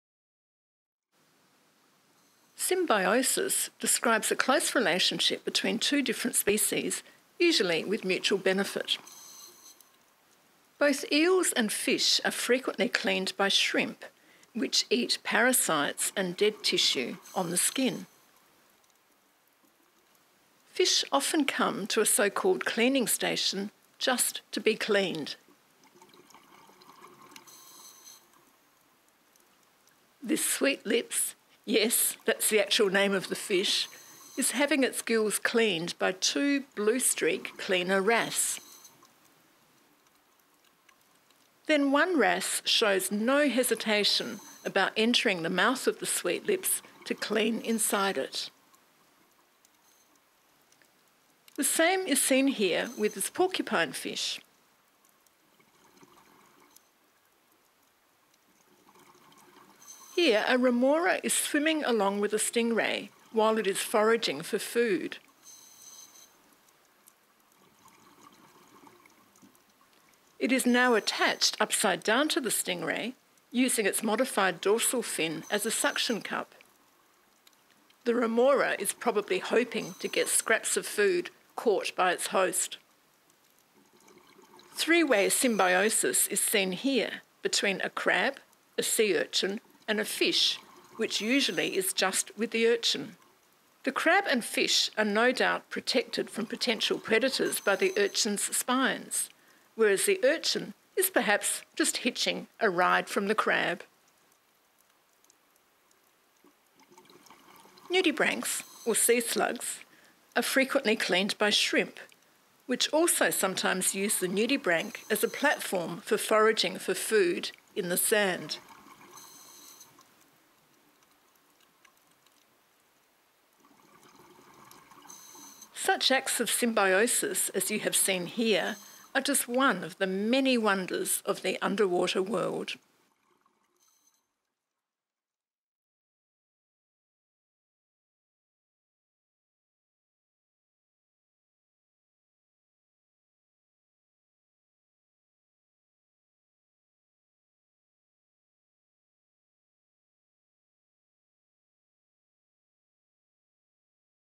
Underwater Symbiosis - Friends with Benefits
Symbiosis is a mutually beneficial relationship between different species. This video shows examples of symbiotic relationships in the underwater world.